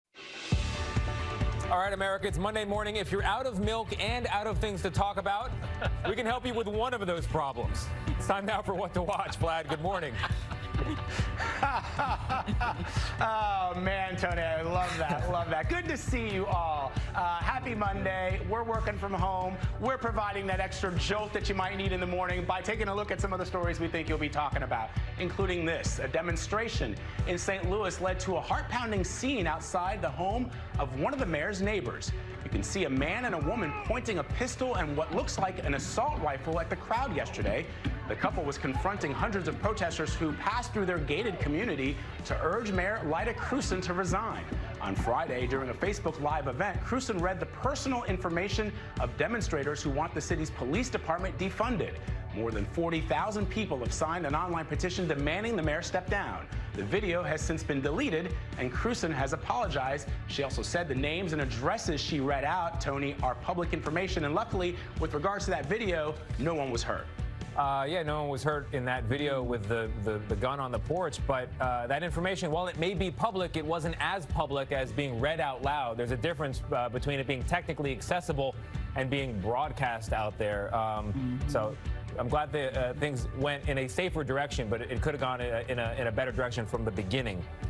St. Louis couple points guns at protesters near mayor's home
A demonstration in St. Louis led to a heart-pounding scene outside the home of one of the mayor's neighbors. A man and woman pointed a pistol and what appears to be an assault rifle at the crowd Sunday. The couple was confronting hundreds of protesters who passed through their gated community to urge Mayor Lyda Krewson to resign.